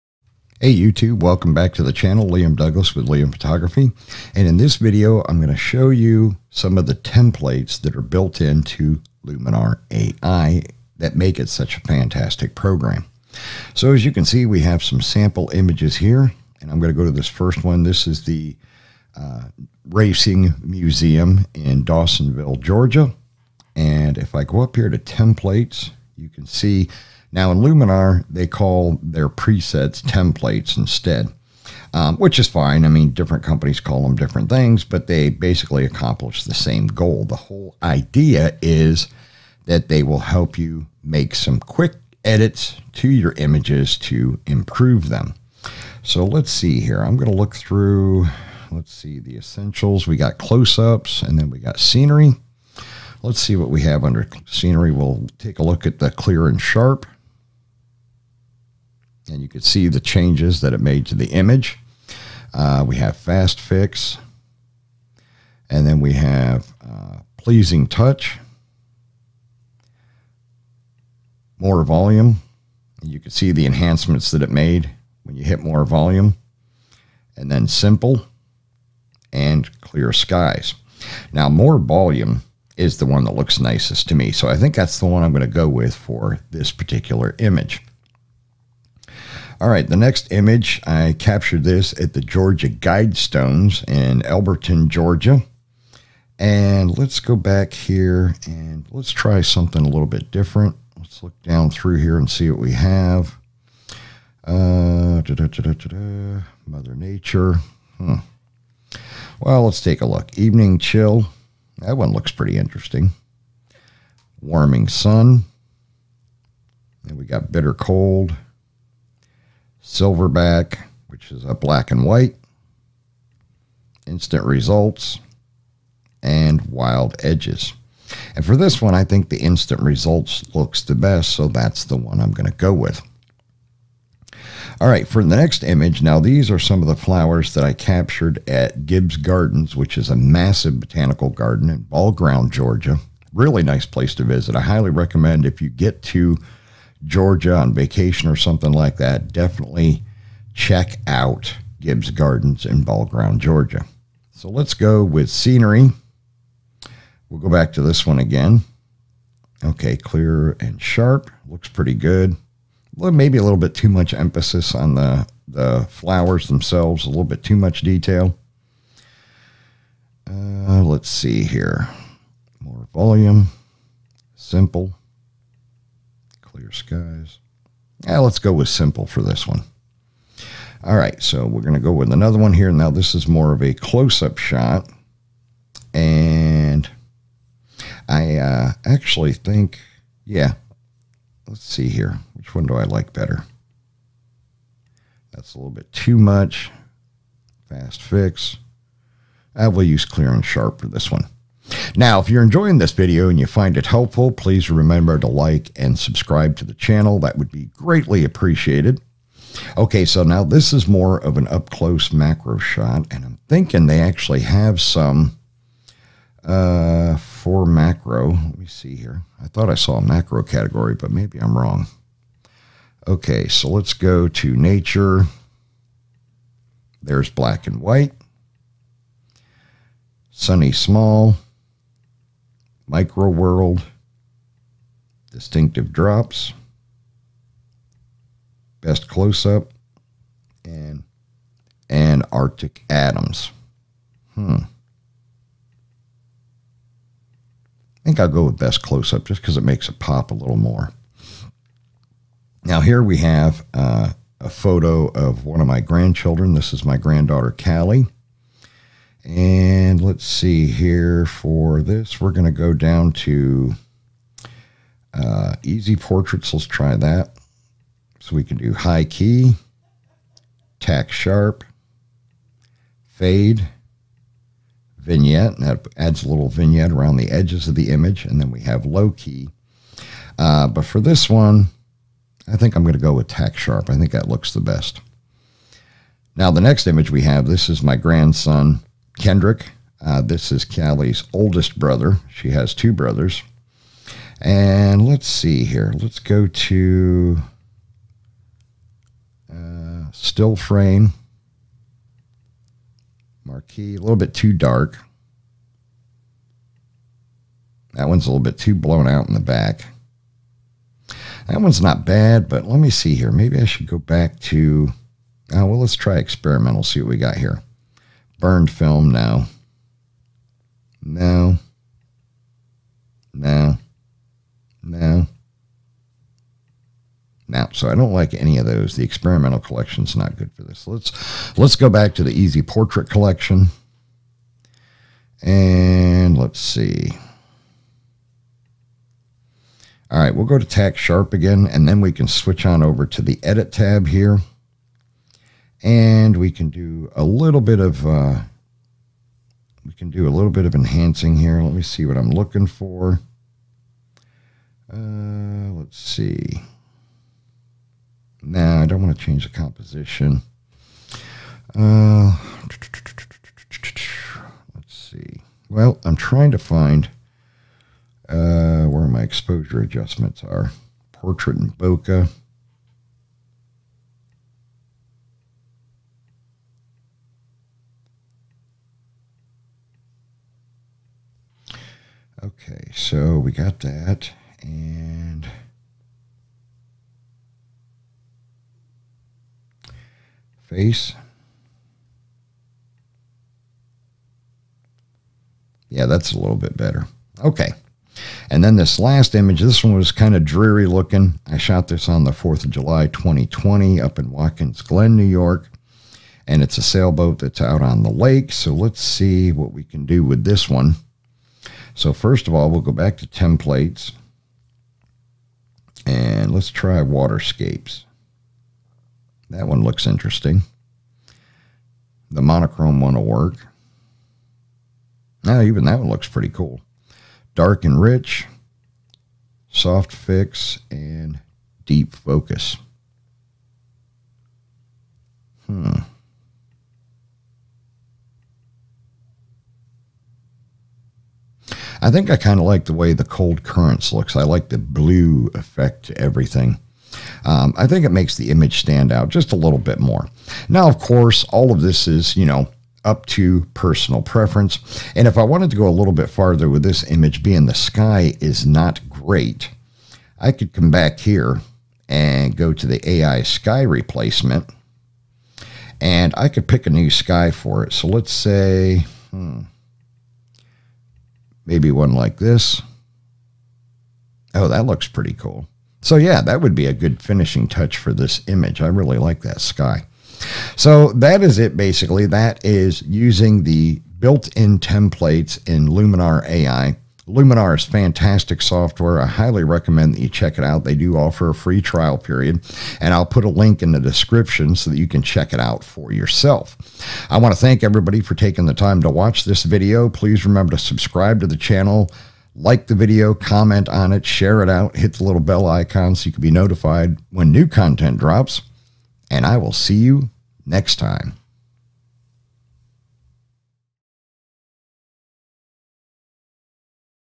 Using Luminar AI Templates
In this video I walk you through using Luminar AI Templates to make some great quick edits to your images.

If you want the best tool for growing your Youtube Channel check out TubeBuddy!